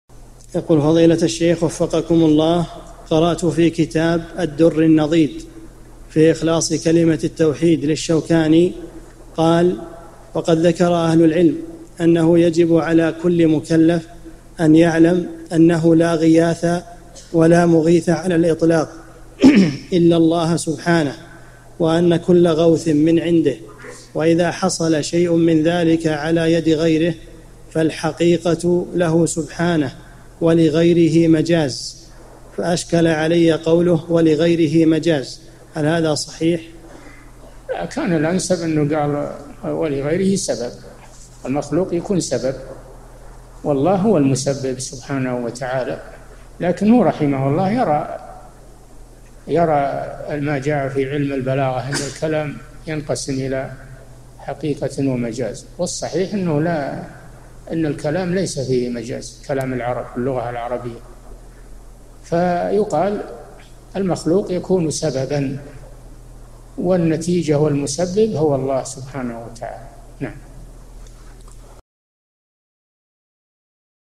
[752 /1350]ذكر الشوكاني أن كل غوث من عنده سبحانه وإذا حصل على يد غيره فالحقيقة له سبحانه ولغيره مجاز
-[752 /1350] السؤال: قرأت في كتاب " الدر النضيد في إخلاص كلمة التوحيد " للشوكاني قال: " وقد ذكر أهل العلم أنه يجب على كل مكلفٍ أن يعلم أنه لا غياث ولا مغيث على الإطلاق إلا الله سبحانه، وأن كل غوثٍ من عنده، وإذا حصل شيء من ذلك على يد غيره فالحقيقة له سبحانه ولغيره مجاز "، فأشكل علي قوله: " ولغيره مجاز "، هل هذا صحيح؟
 الجواب: كان الأنسب إنه قال: "ولغيرهِ سبب"، المخلوق يكون سبب، والله هو المُسبب سبحانه وتعالى، لكن هو رحمه الله يرى أن ما جاء في علم البلاغة أن الكلام ينقسم إلى حقيقةٍ ومجاز، والصحيح أن الكلام ليس فيهِ مجاز، كلام العرب، اللغة العربية. فيُقال: المخلوق يكونُ سبباً، والنتيجة والمُسبب هو الله سبحانه وتعالى.
للاشتراك في قناة الشيخ صالح الفوزان
أسئلة وأجوبة من شرح قاعدة جليلة ( 1350 ) مقطع

للاستفسار والإقتراحات والدعم
رئيس الفريق
لمشروع كبار العلماء